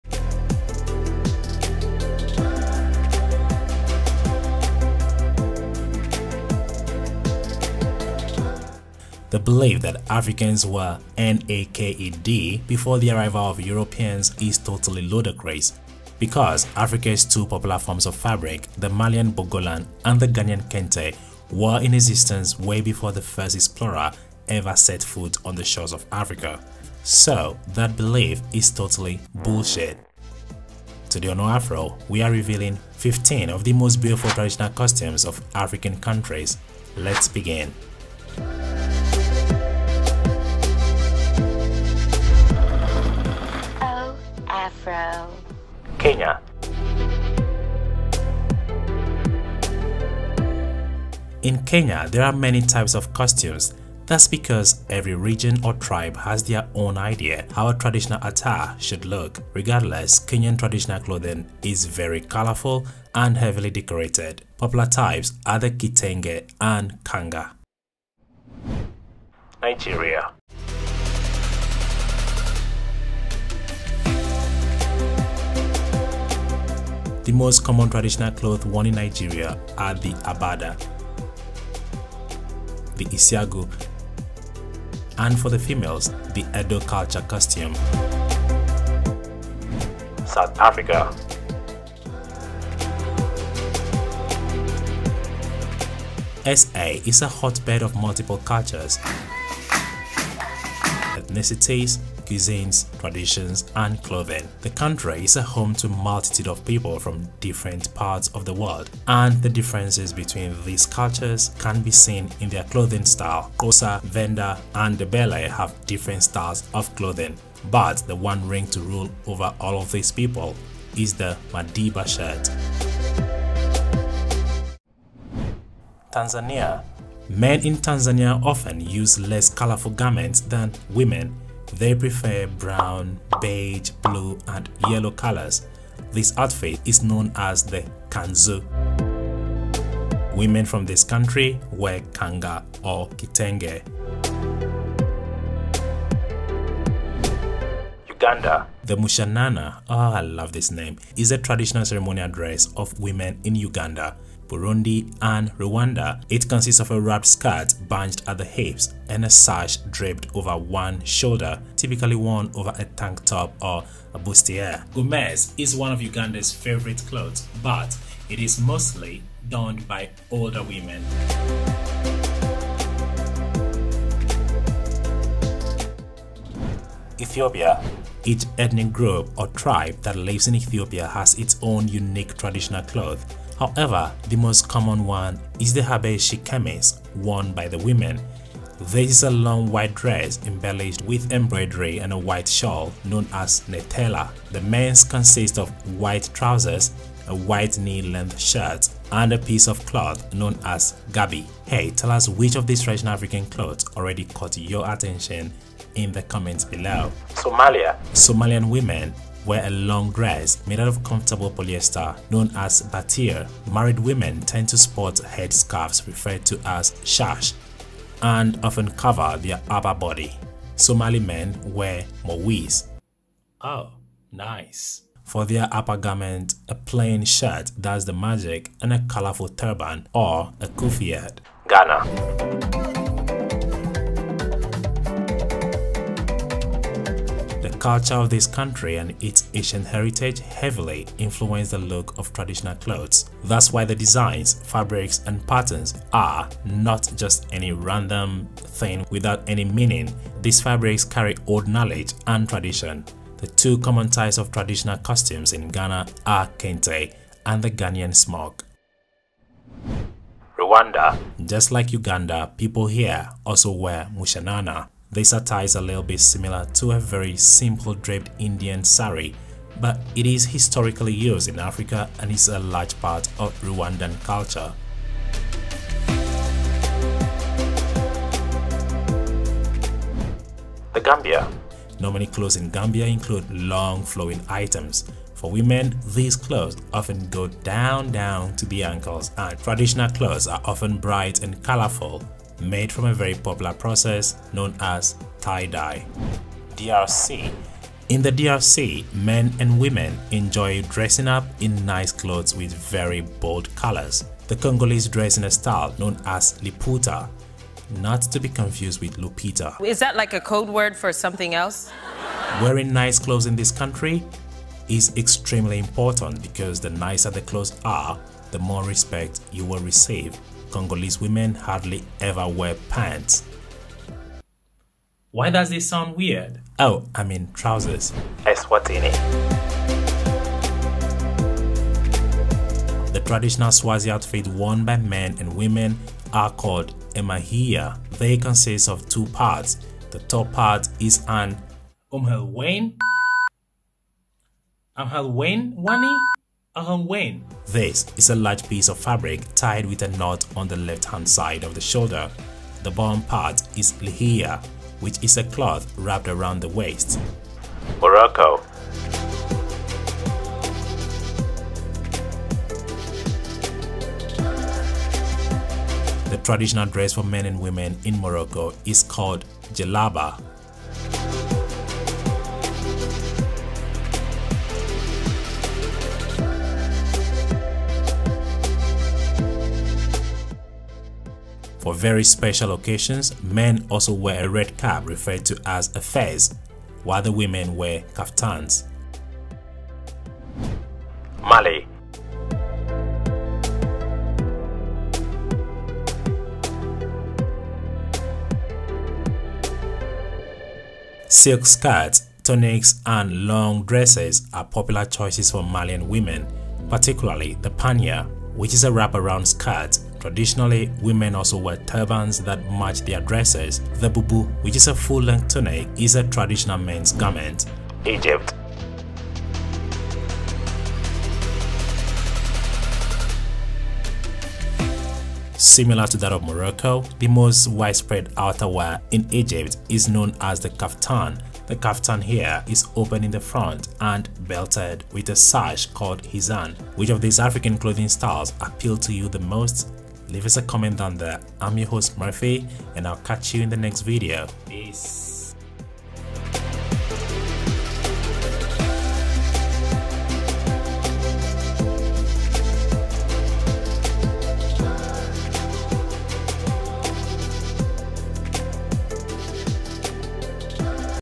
15 Super Beautiful African Traditional Clothes
We have cited examples of Beautiful African clothes from 15 African countries : Egypt, Mali, Morocco, Eswatini DRC, Gambia, Rwanda, Ghana, Somalia, Ethiopia, Uganda, Tanzania, South Africa, Nigeria and Kenya. This African costume are used as daily wears, for occasions suck as marriages and parties. there are many other countries in Africa that offer a wide variety of and beautiful costumes. Enjoy!

15 Beautiful African Costumes
0:00 Intro
0:46 Kenya
1:09 Nigeria
1:29 South Africa
2:05 Tanzania
2:28 Uganda
2:58 Ethiopia
3:27 Somalia
3:50 Ghana
4:20 Rwanda
4:40 Gambia
4:56 D.R.C
5:28 Eswatini
5:57 Morocco
6:32 Mali
6:57 Egypt